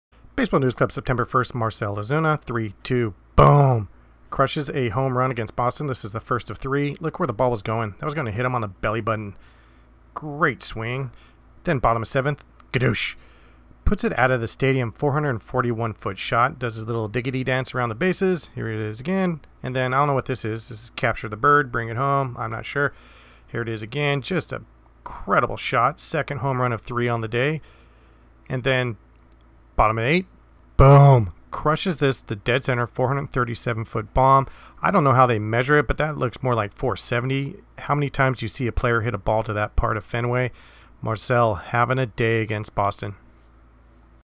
Marcell Ozuna (3) HR day vs Boston RedSox 9.1
** Marcell 3 for 5 with (3) HR's, 3 runs and 6 RBI in a beat down on the road in Boston. Braves continue to be competitive and creating separation in the NL East.